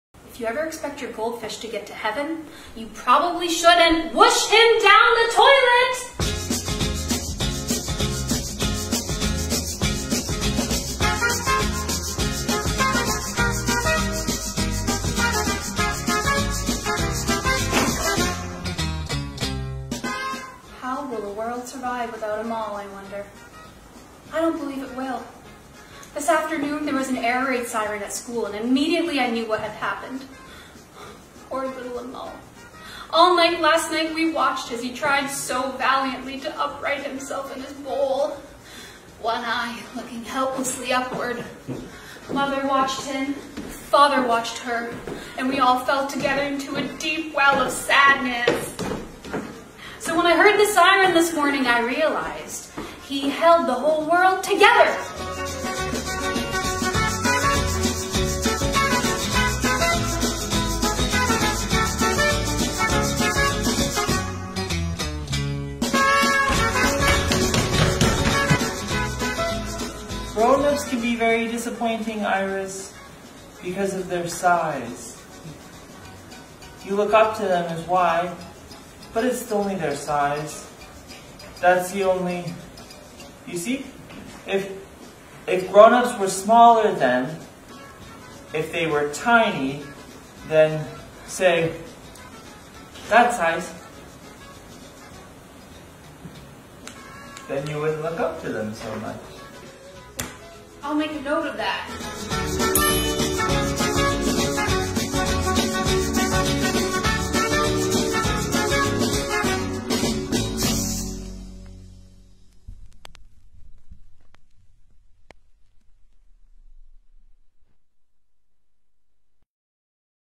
Girl in the Goldfish Bowl
Clips from Langham Court Theatre's 2017 production of Girl in the Goldfish Bowl written by Morris Panych and directed by Janet Munsil.

Ten-year-old Iris struggles with with the death of her fish, Amahl, and the quirkiness and dysfunction of her family while growing up in 1960s Steveston, BC.

Creative Team:
Director: Janet Munsil
Set Designer: Don Keith
Lighting Designers: Carol Anne Moore and Paul Hilton
Sound Designer: Jason King
Properties and Set Decor: Linda MacNaughton
Stage Manager: Euphemia MacMurchy

Cast:
Iris: Lianne Coates
Mr Lawrence: John Manson
Sylvia: Connie McConnell
Owen: Wayne Yercha
Miss Rose: Hillary F Allan